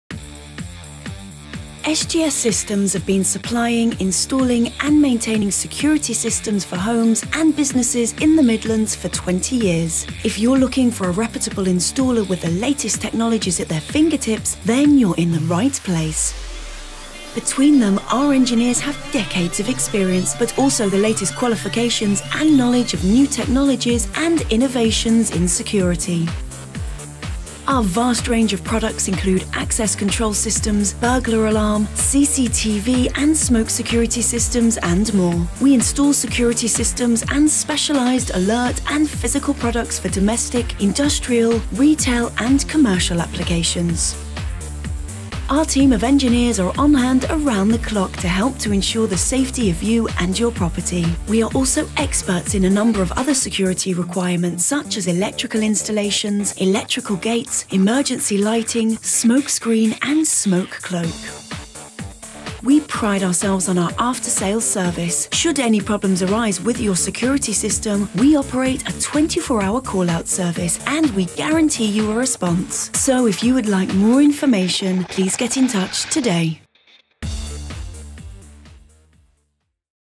Burglar Alarms & Security Systems - S.G.S Systems Ltd
Protect your business premises with a range of effective solutions supplied, installed, and maintained by our team of qualified engineers. Rest assured that your business is safe in our hands, even when your staff have left for the day.